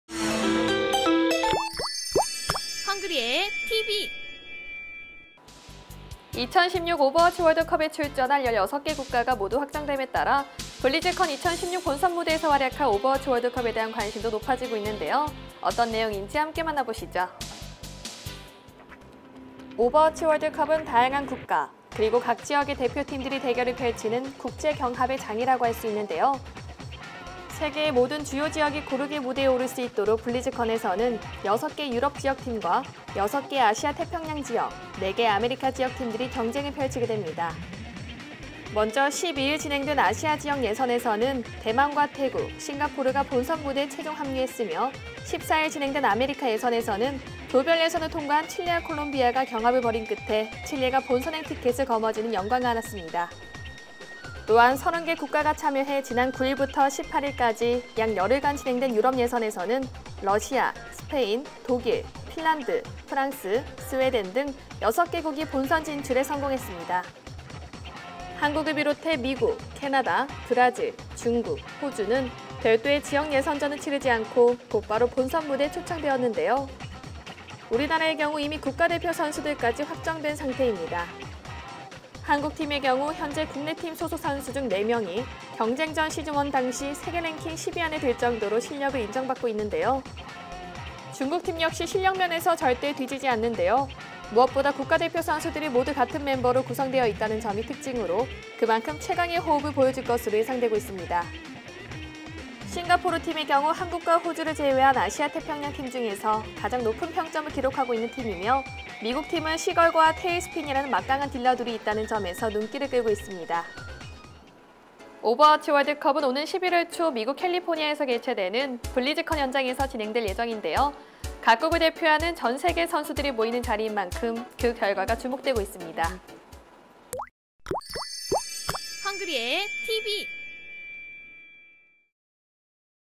오버워치 월드컵 출전 16개국 확정! 국가별 전력은?!
오버워치 월드컵에 출전할 16개국이 모두 확정됐다. 오는 11월 초
미국 캘리포니아에서 개최되는 블리즈컨 2016 현장에서 활약할
국가들이 확정됨으로써 오버워치 월드컵에 대한 관심도 높아지고 있다